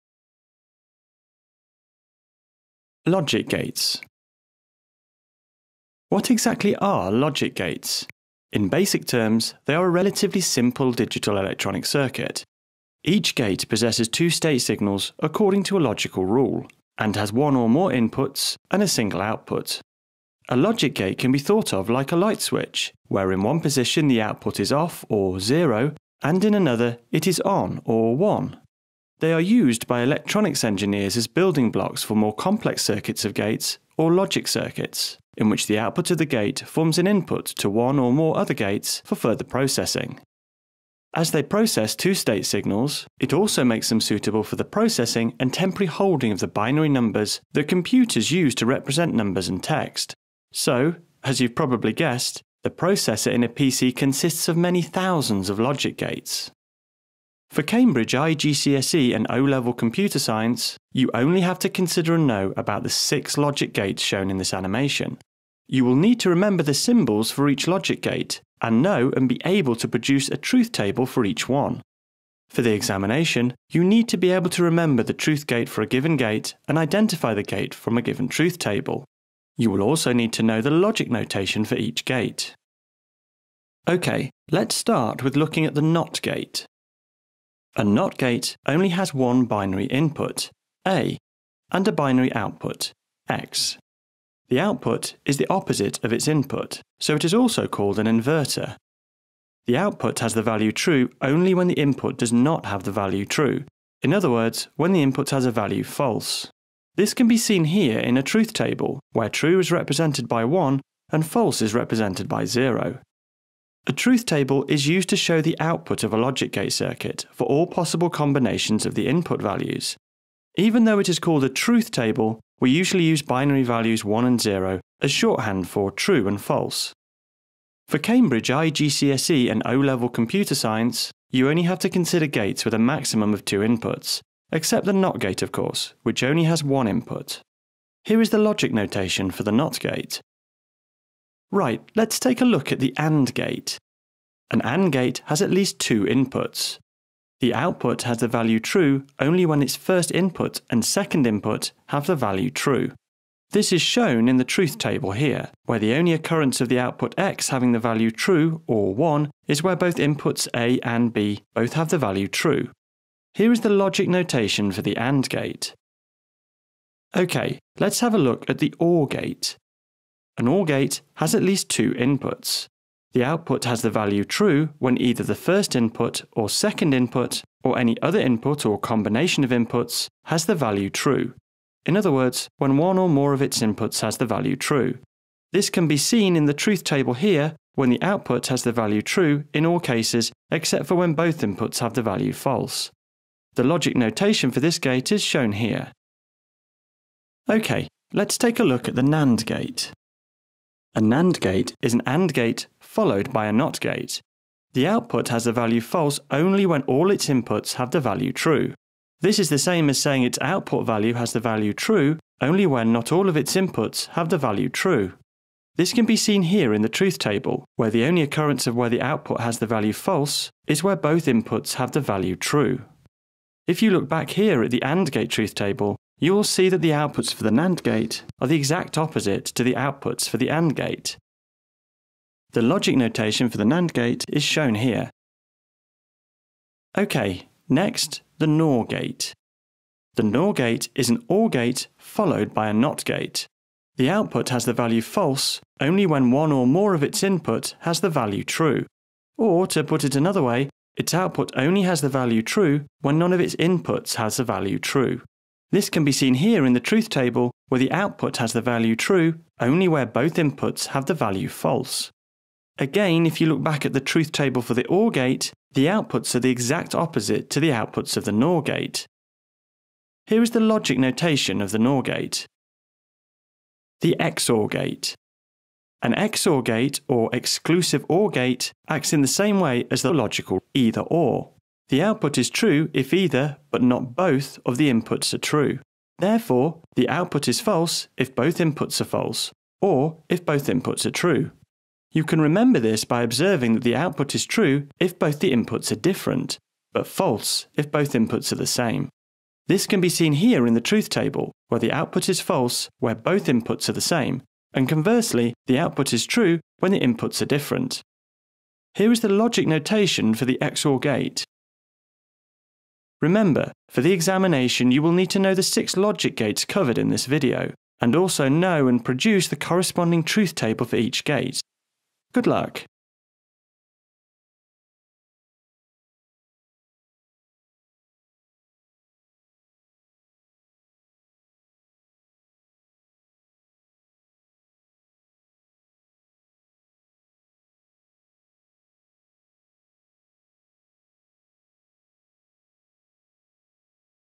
Logic gates (Cambridge O levels Computer Science)
Logic gates, what exactly are they? In basic terms, they are a relatively simple digital electronic circuit. Each gate processes two-state signals according to a logical rule, and has one or more inputs and a single output. A logic gate can be thought of like a light switch, where in one position the output is off or zero, and in another, it is on or one. They are used by electronics engineers as building blocks for more complex circuits of gates or logic circuits, in which the output of a gate forms an input to one or more other gates for further processing.
As they process two-state signals, it also makes them suitable for the processing and temporary holding of the binary tnumbers that computers use to represent numbers and text. So, as you have probably guessed, The processor in a PC consists of many thousands of logic gates. For Cambridge IGCSE and O Level Computer Science, you only have to consider and know about the six logic gates shown in this animation. You will need to remember the symbols for each logic gate, and know and be able to produce a truth table for each one. For the examination you need to be able to remember the truth table for a given gate and identify a gate from a given truth table. You will also need to know the logic notation for each gate.